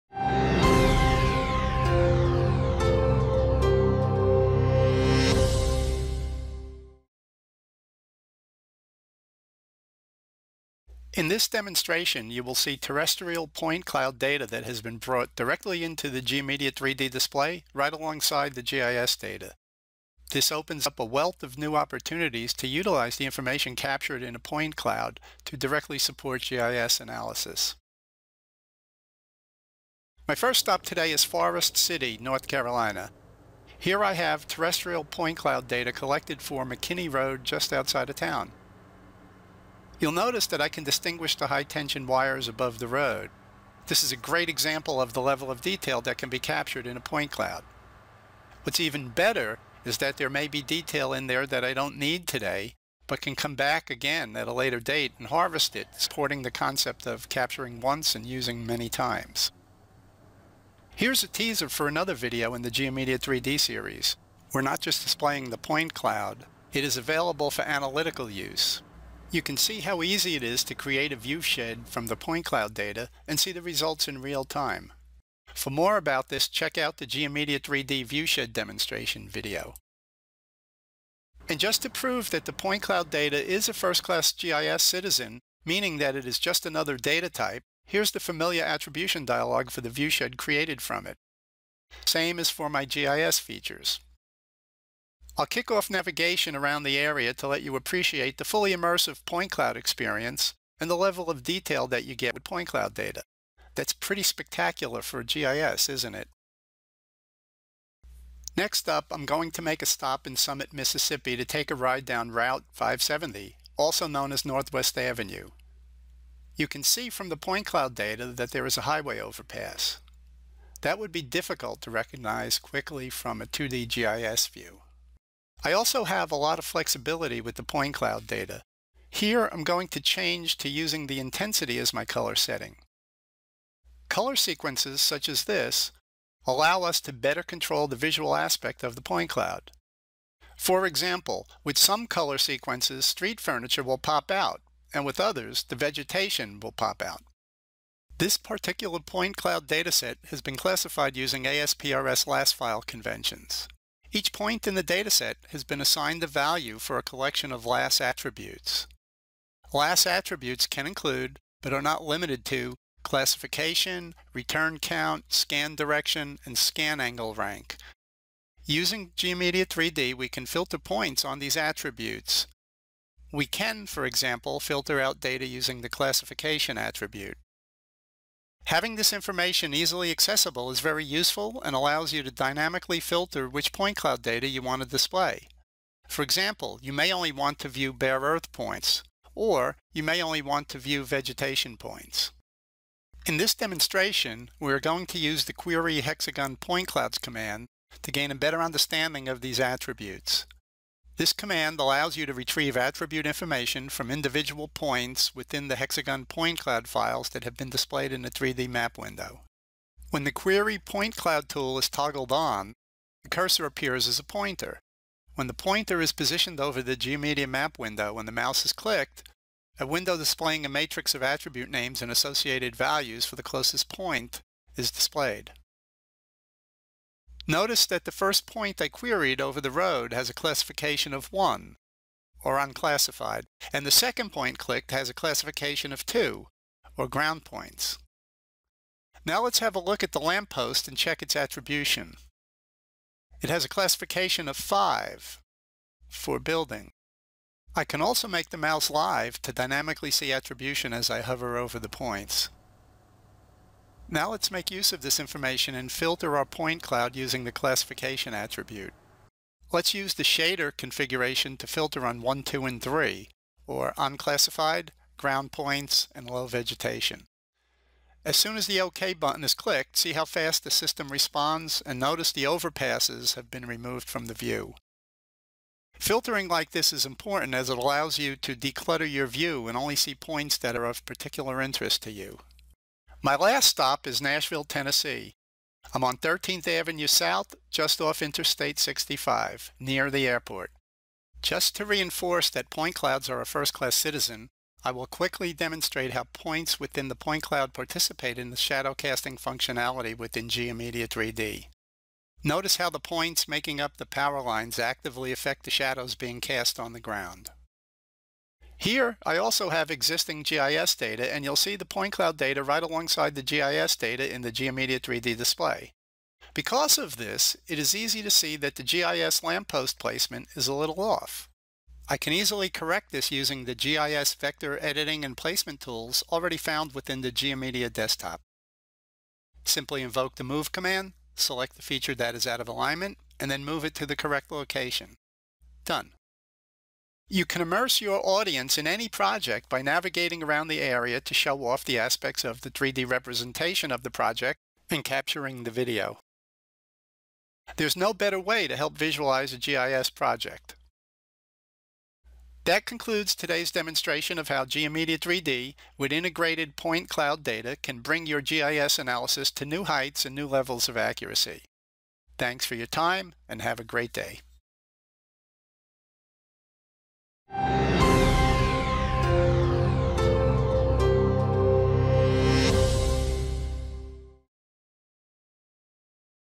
GeoMedia 3D Point Cloud Integration Demo
You can now bring terrestrial point cloud data directly into GeoMedia 3D (an extension to GeoMedia) and display it right alongside the GIS data. This opens up a wealth of new opportunities to utilize the information captured in a point cloud to directly support GIS analysis.